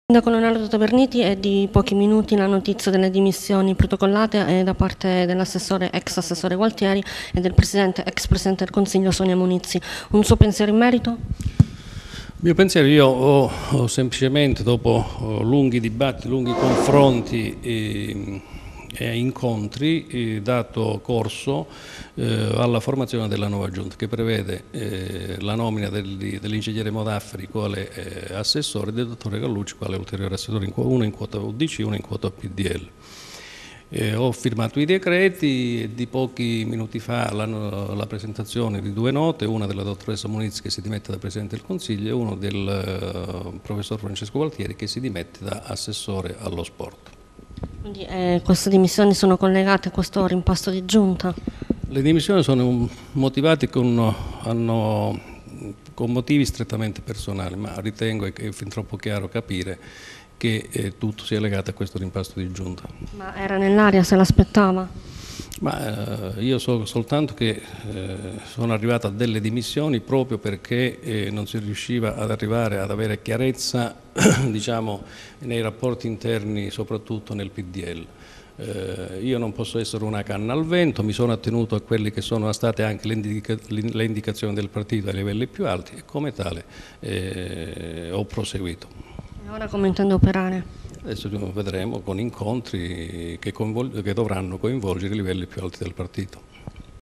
19/04/2013 - Soverato - Il Sindaco sul rimpasto e le dimissioni di Munizzi e Gualtieri
19/04/2013 - Soverato - Il Sindaco sul rimpasto di Giunta odierno e le dimissioni di Sonia Munizzi e Francesco Gualtieri ... Servizio di Antonella Rubino